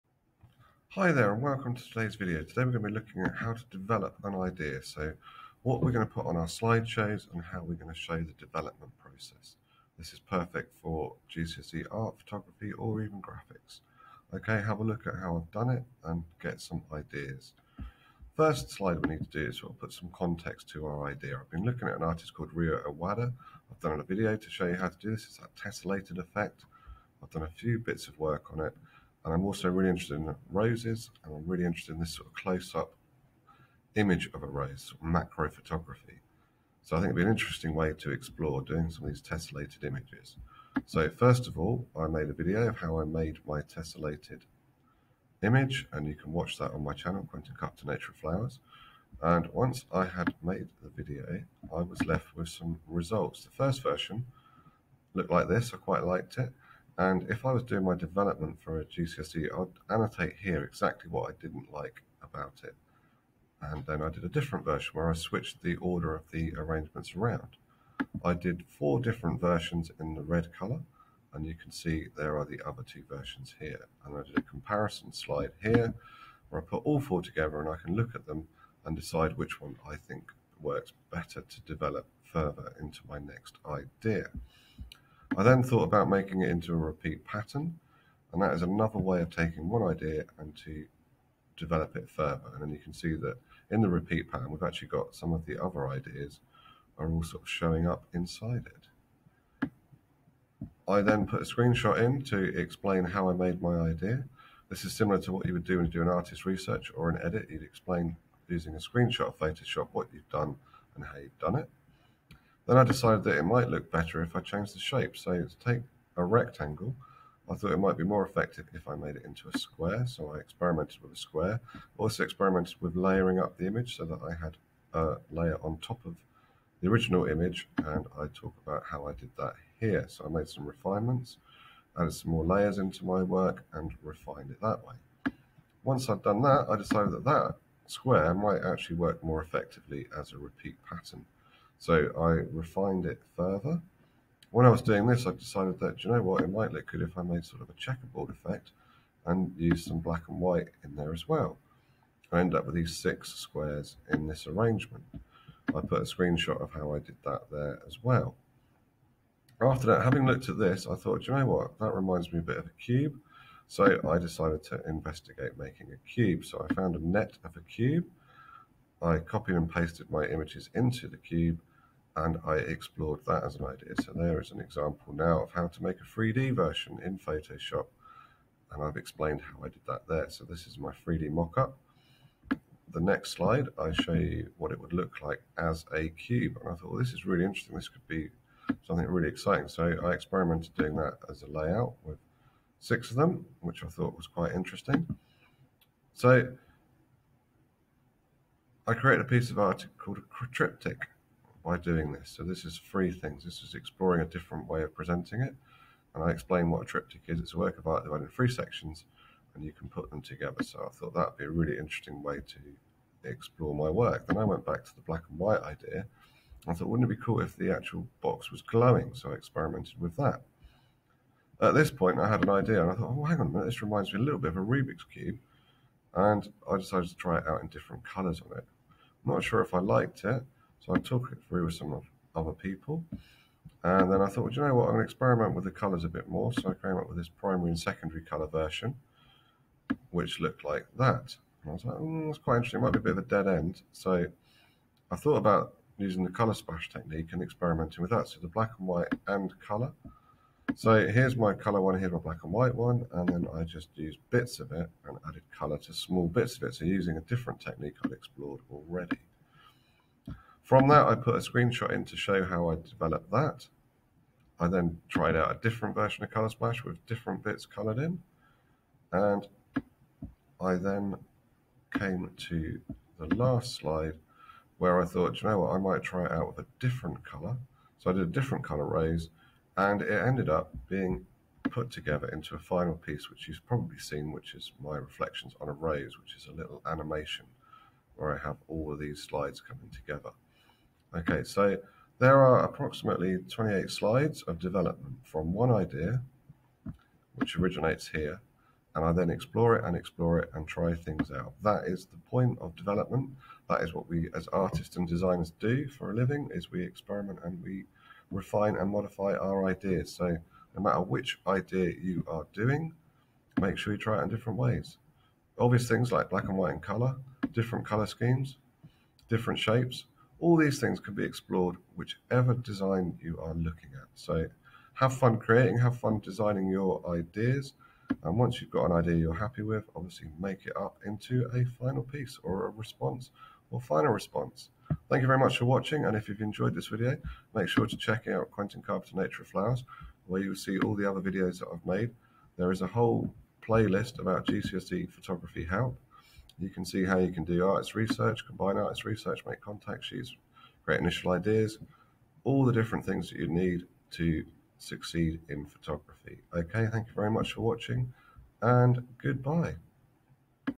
How to Develop Ideas using Google Slides
In this video I show you how to use GoogleSlides to show your Development of Ideas for GCSEPhotography.
In this example I have looked at Ryo Ohwada and I think this could be an interesting way of modifying and adapting my flower photographs to give them a more wider appeal. I show you lots of different ways to modify, adapt and refine this idea and explore them in detail.
Have fun taking your ideas and developing them further.